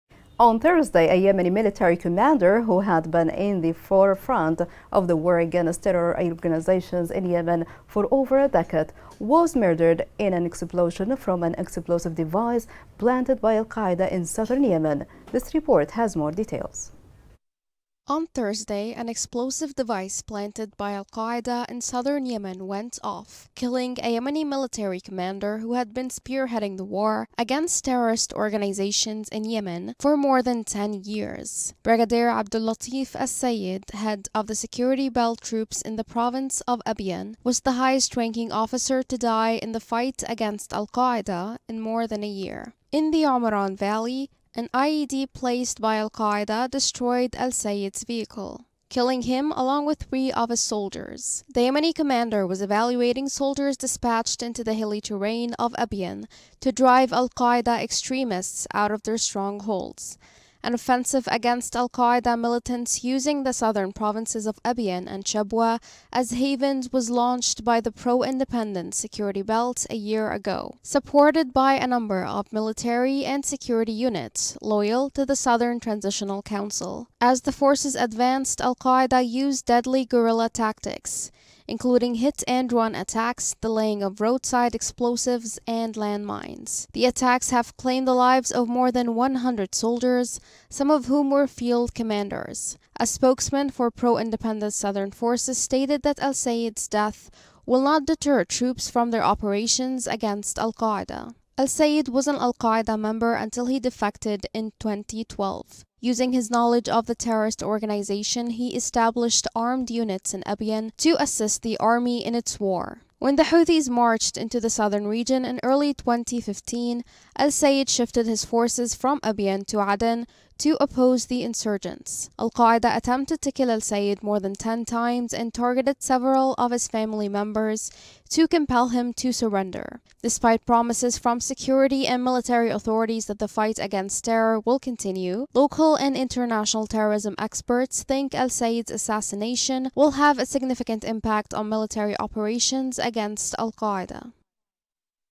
Al-Qaeda Faces Condemnation for Assassinating Security Belt Forces Commander
Like Yemen Today NEWS on Facebook